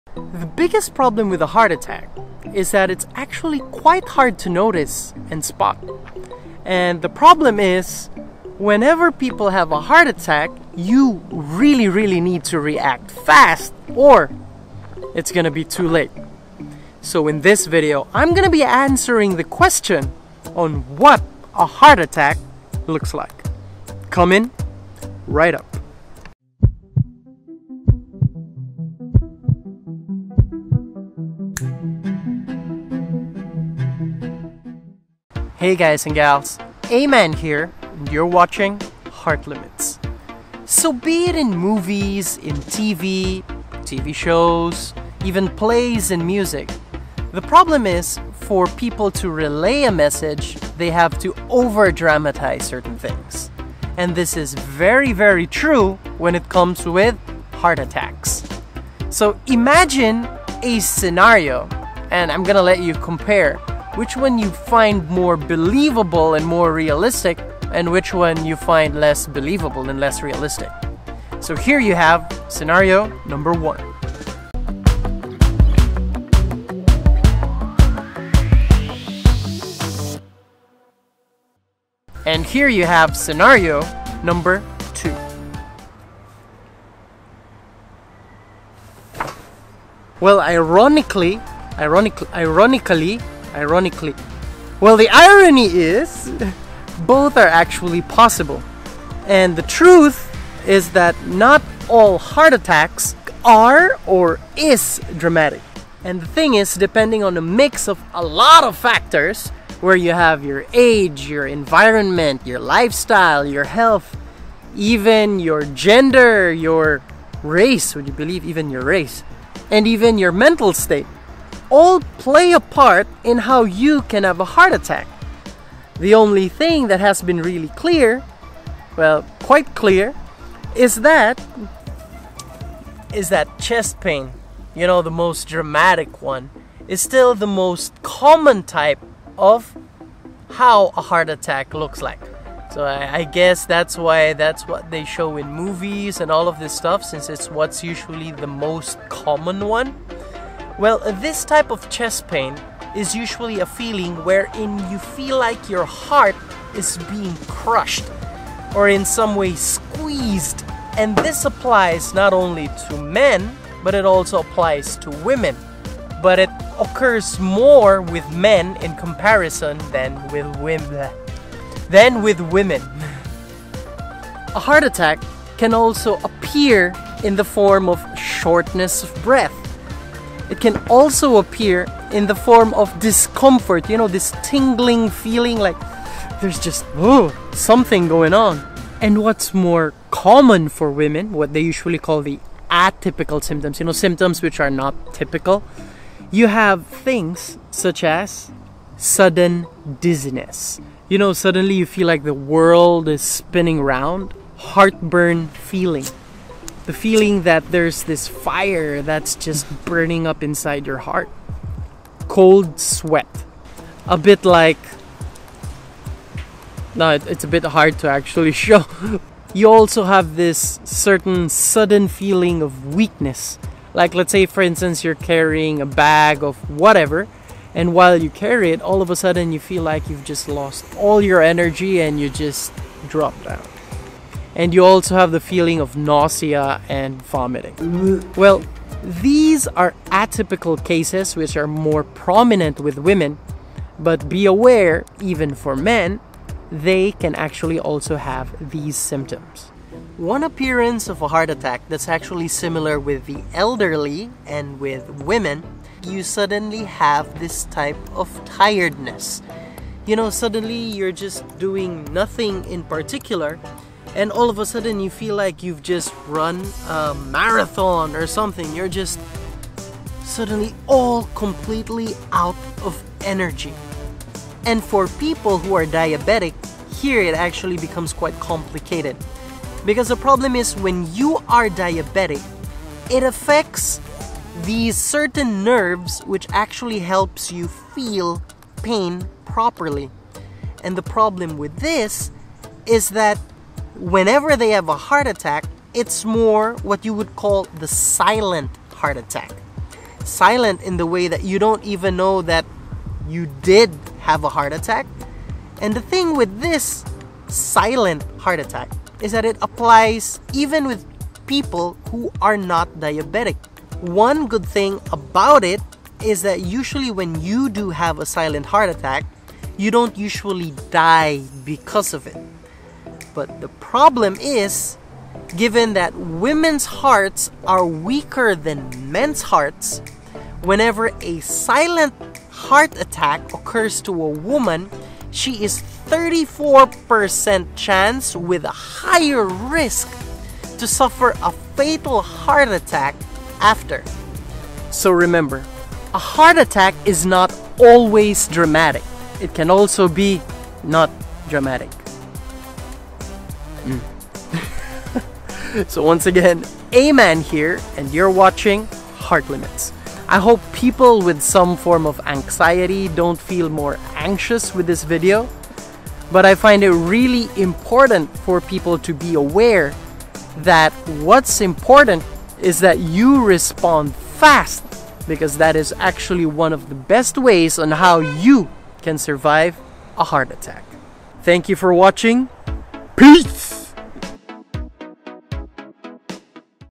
How Heart Attacks Look For Men And Women
In this video, Heart Limits shows the difference in how heart attacks appear between men and women.

My Main and Vlog Camera
My FAVORITE Lens Ever
The Mic I use
Best Tripod ever

Location of shoot: Bordeaux, France

About this video: In this video Aman from Heart Limits tries to clearly explain the typical and atypical symptoms of a heart attack for both men and women. The goal of this video is to promote awareness and to give people hints on when they should call for medical help fast.